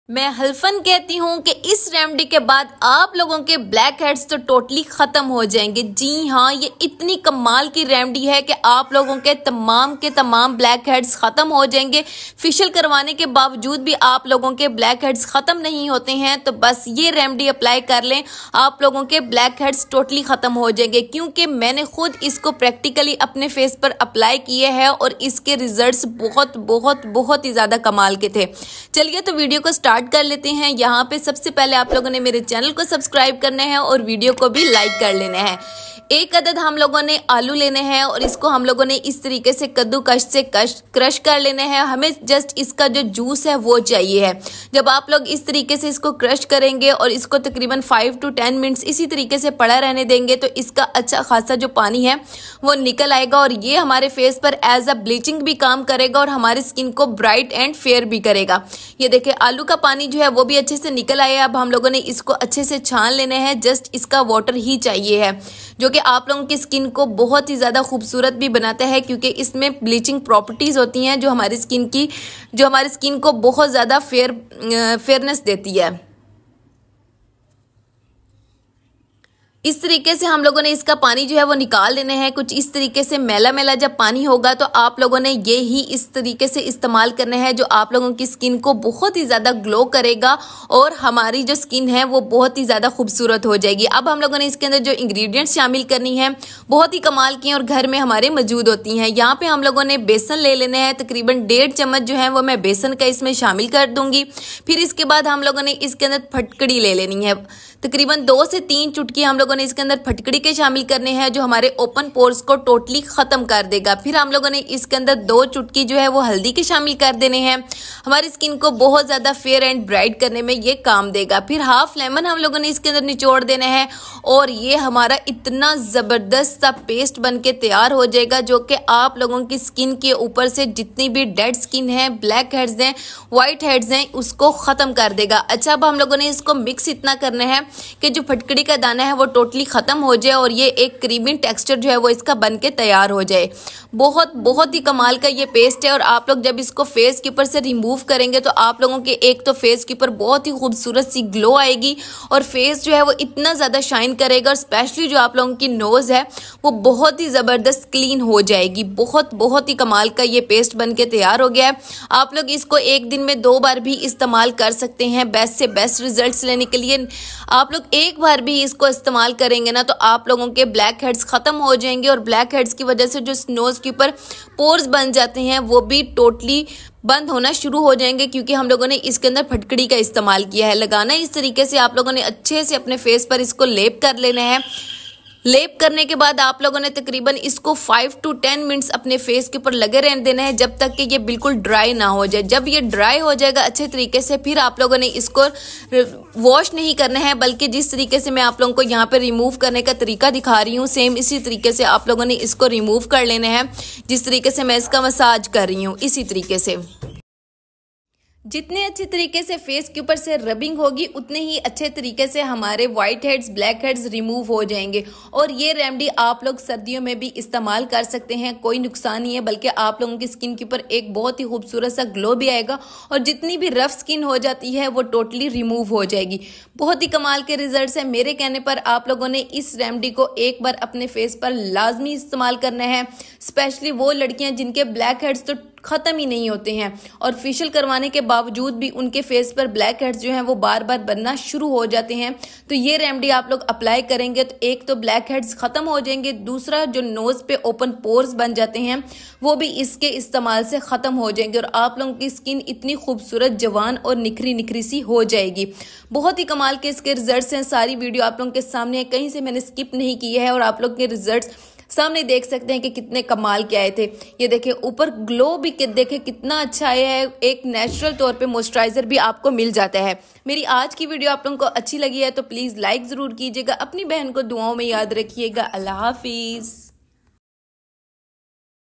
How To Remove Black Head & White Head At Home | Best Black Head Removal Remedy
Aoa Friends,
Today I'm making remedy how to remove black head & white head at home. Best black head removal remedy.

Ingredients:-
√ Potato
√ Gram flour
√ Turmeric powder
√ Alum Powder

Its really effective remedy i personally use this remedy and share my views in this remedy.

Benefits:-
Potato Juice Heals Sun-Damaged Skin. ...
Potato Juice On Face Helps For Brightening Complexion. ...
Potato Juice Combats Blemishes. ...
Applying Potato Juice On Face Helps To Fight Acne. ...
Fights Visible Signs Of Ageing. ...
Potato Juice Moisturises Skin.